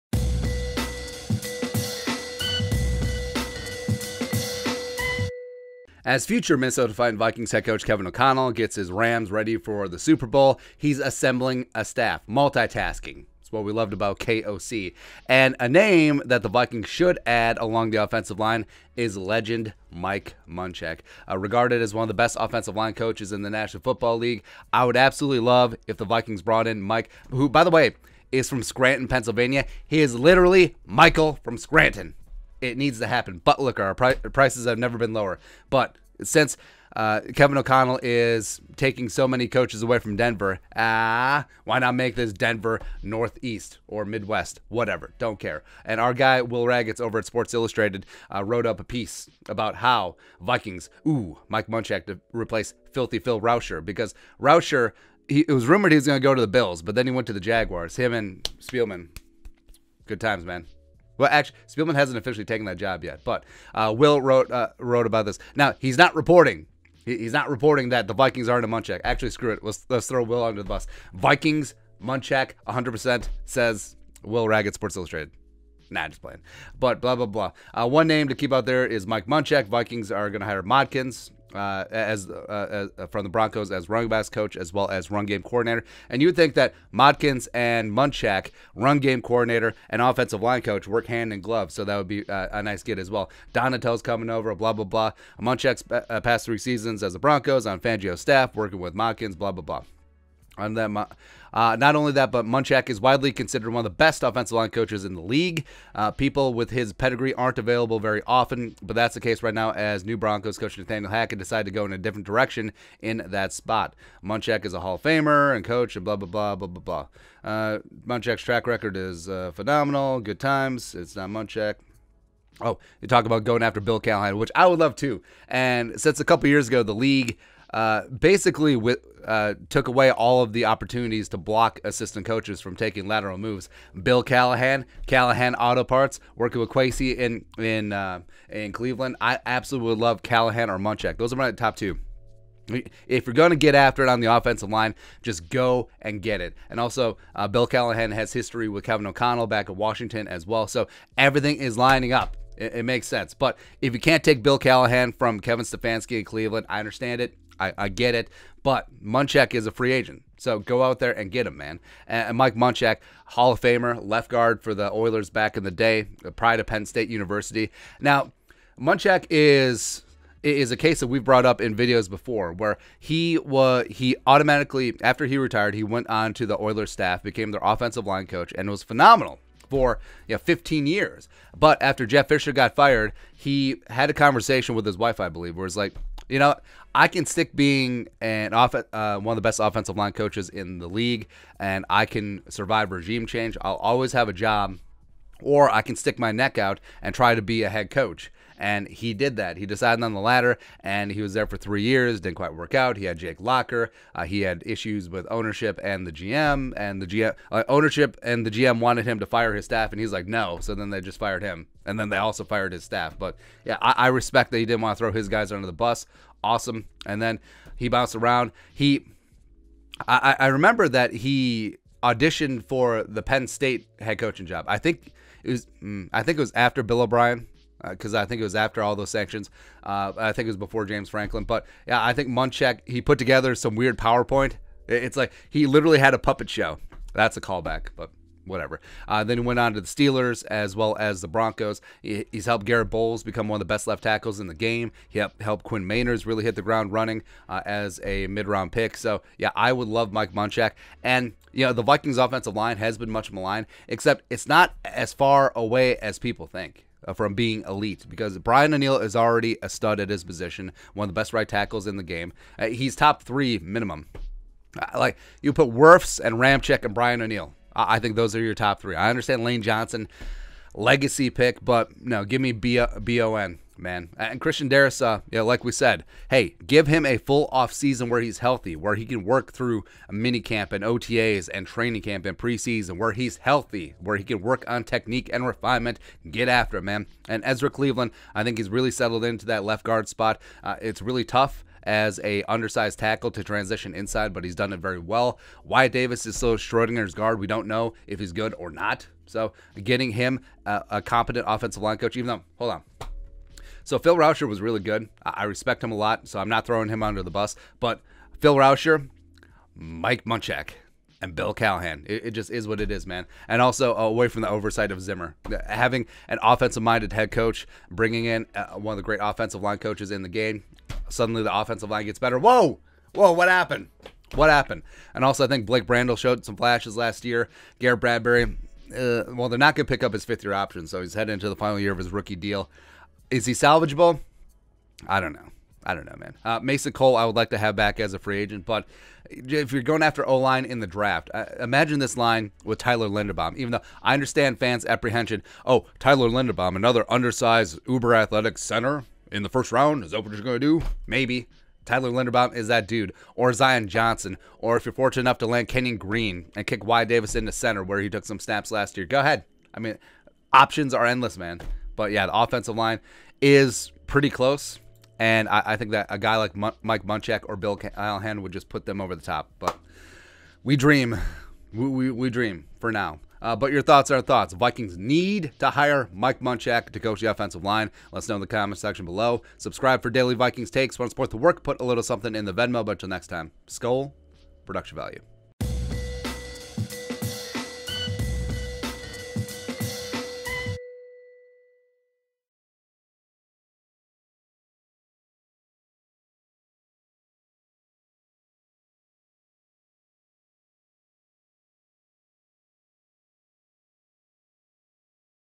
Vikings NEED to Hire O-Line Coaching Legend Mike Munchak
As new Minnesota Vikings head coach Kevin O'Connell begins assembling his first staff, Mike Munchak (one of the best offensive line coaches in the NFL) is just hanging out there. Chillin'. Go get him.